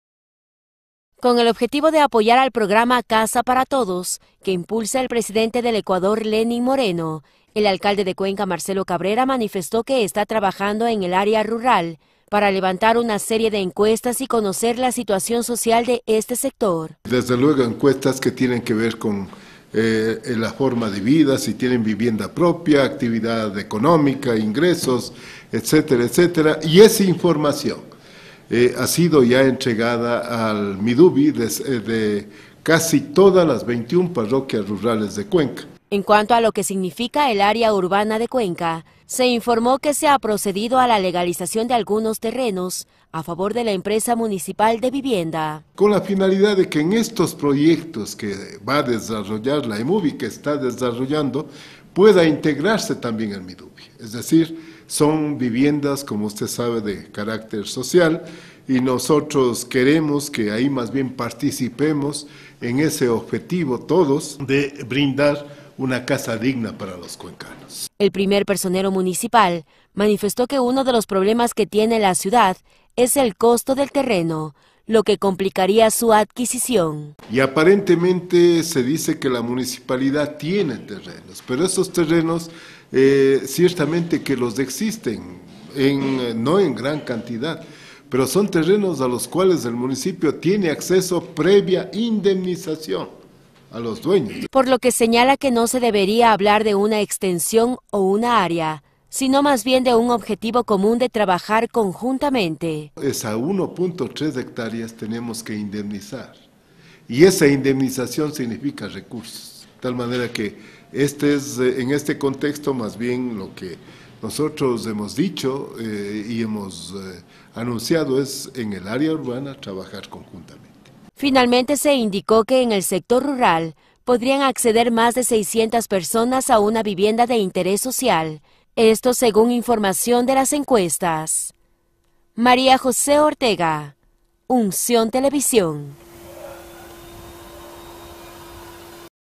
Alcalde de Cuenca apoya el programa Casa para todos
La Municipalidad y el Gobierno Nacional realizan encuestas para conocer la situación del área rural en cuanto al tema habitacional. Esto con miras para el programa “Casa para todos”. María José Ortega con los detalles en la siguiente nota.